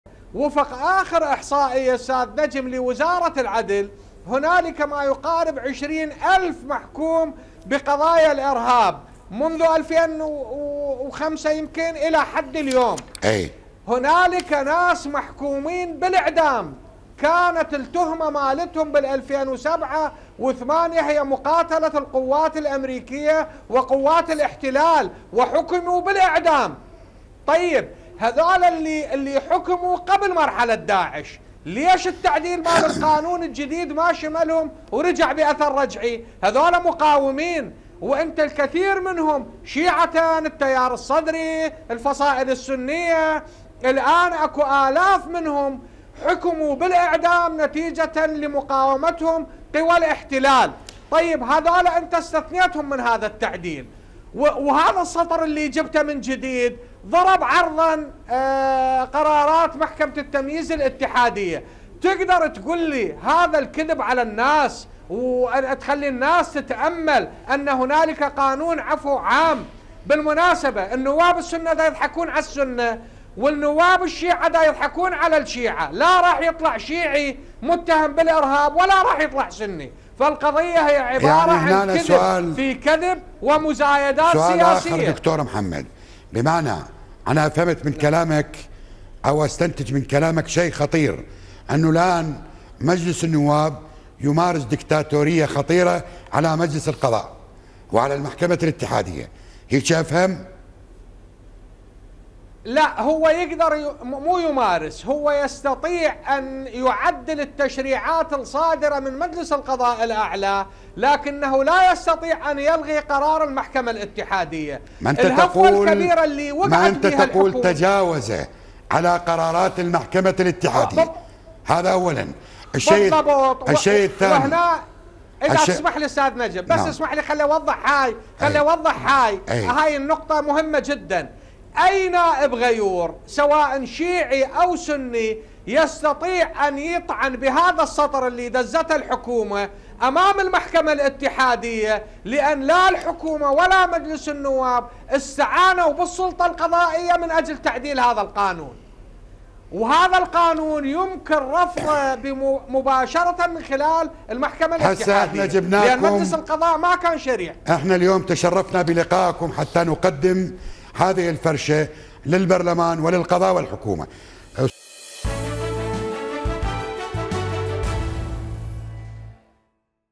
نائب عراقي سابق يستنكر عدم شمول "من حارب أمريكا" بقانون العفو العام
موقع قناة التغيير الفضائية  الالكتروني